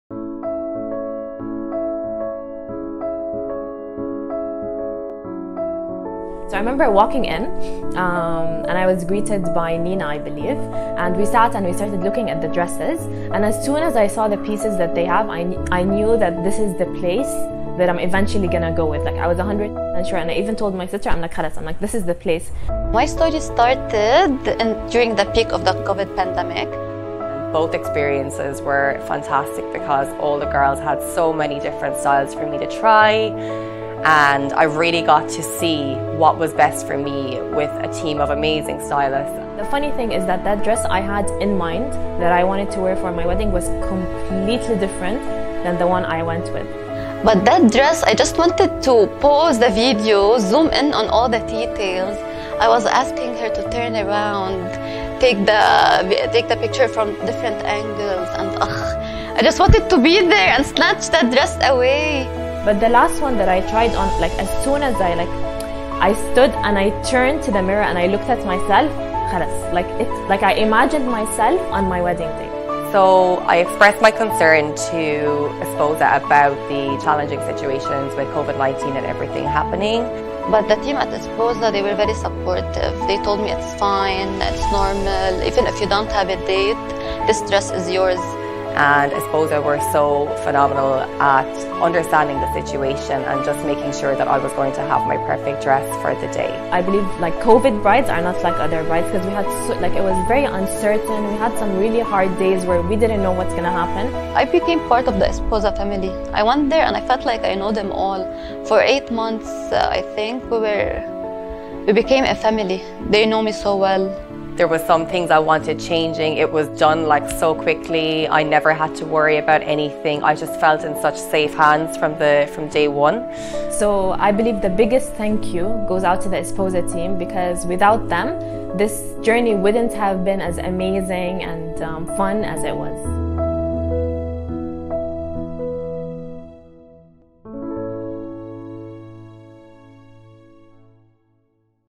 Bridal Testimonials - COVID Brides | تجربة العروس في اسبوزا خلال كورونا
Esposa Group tells the ultimate love stories through the experiences and testimonials of each bride we welcomed into our family ! This round of bridal testimonials is special as it tells the experiences of these brides during the pandemic and the struggles and uncertainty they went through throughout this experience.

تروي اسبوزا قصص الحب الرائعة من خلال تجارب وشهادات كل عروس انضمّت الى عائلتنا! هذه الجولة من شهادات العرائس مُميّزة لأنها تخبرنا عن تجارب  العرائس في ظلّ جائحة كورونا والصراعات التي مررن بها طوال هذه الفترة الصعبة.